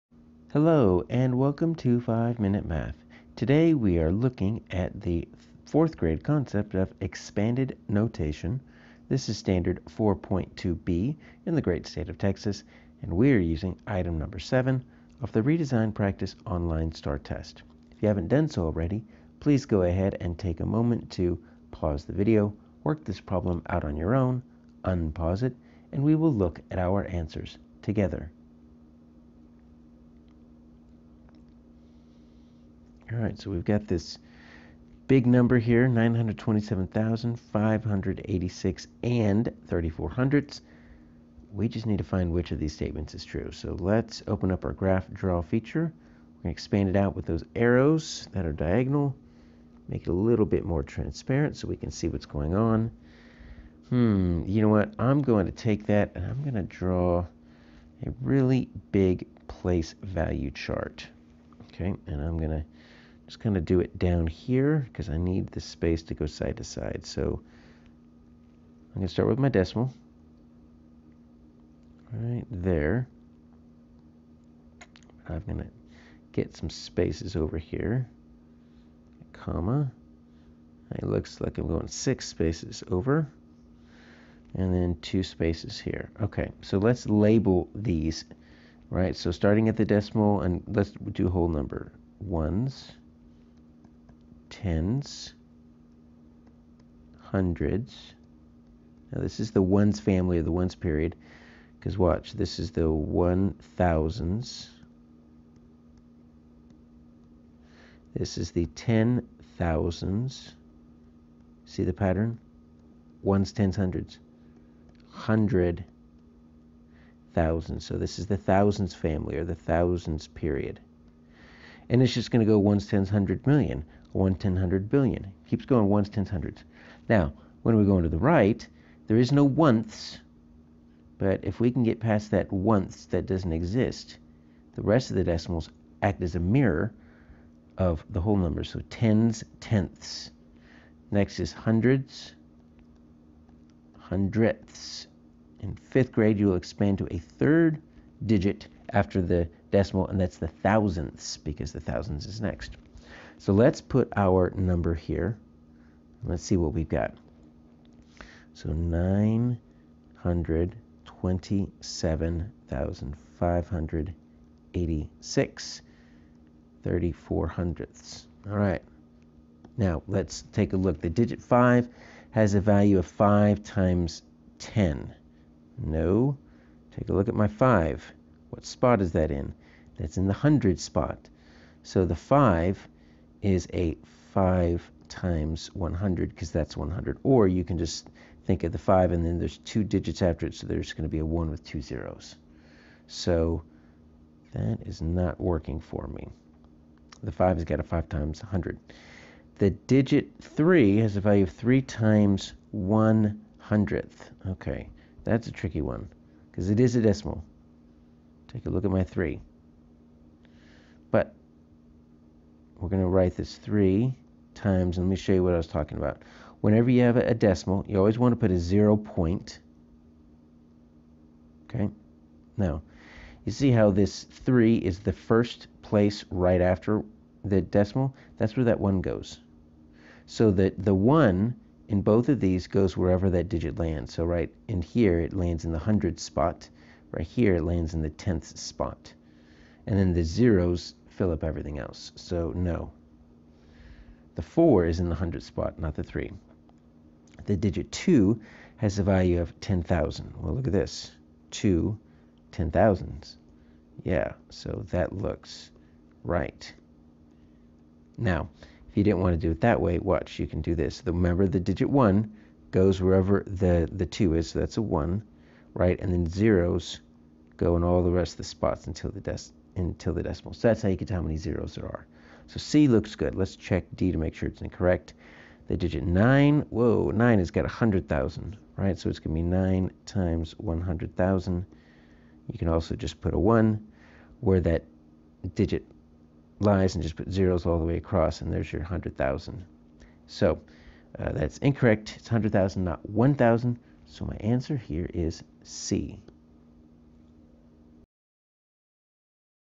4th Grade STAAR Practice Expanded Notation (4.2B - #13)
Practice representing the value of the digit in whole numbers through 1,000,000,000 and decimals to the hundredths using expanded notation and numerals with a test item from the redesign practice STAAR test (#7 - 4.2B).